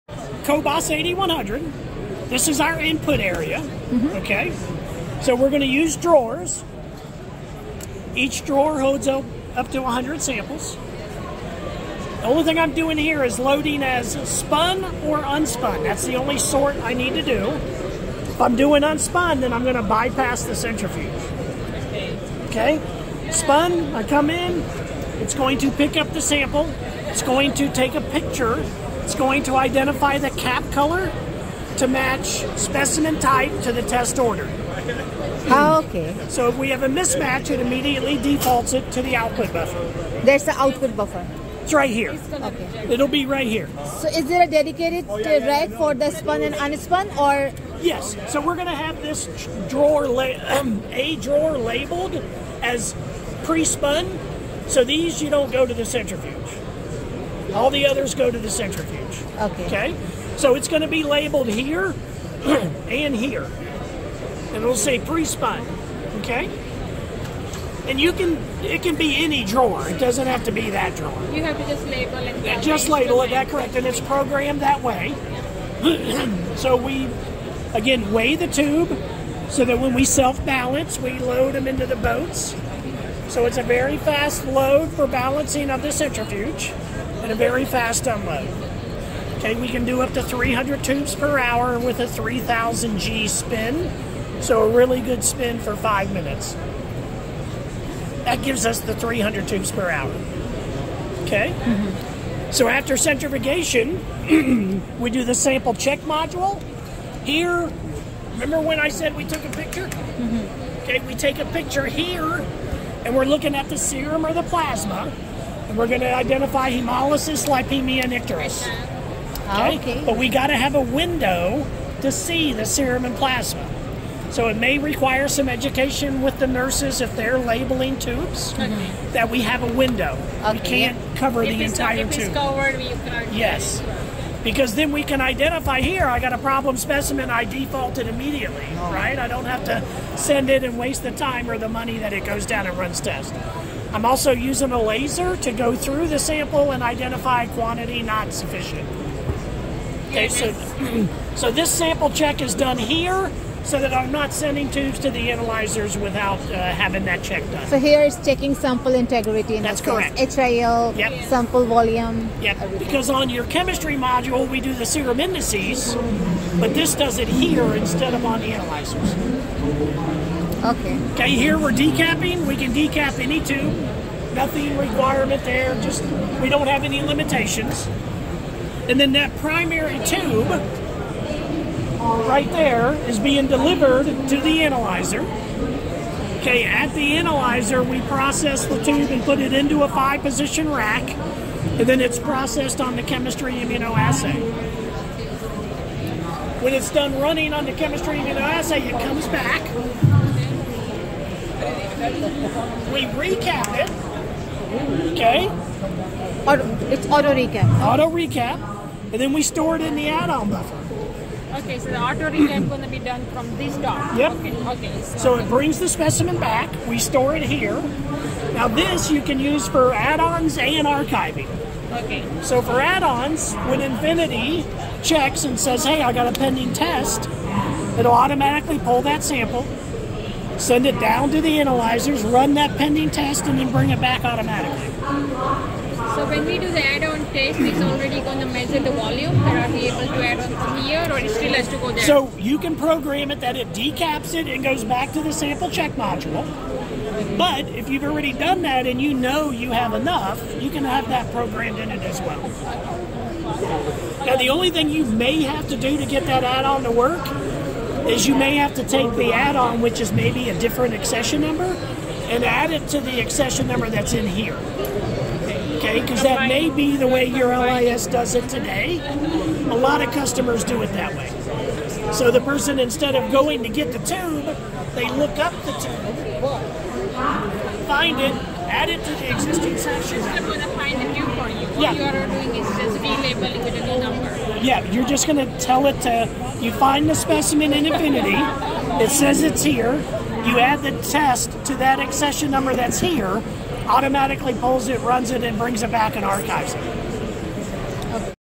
Roche Cobas 8100 preanalytical system, highlighting its features & functions| Clinical Lab Expo
Australian National Science Week: Exploring Innovations in Medical Laboratory Technology
In celebration of Australian National Science Week, I am thrilled to share insights from the recent conference where I had the opportunity to engage with leading vendors in the field of medical laboratory technology. These discussions and demonstrations provide a glimpse into the cutting-edge innovations shaping our industry.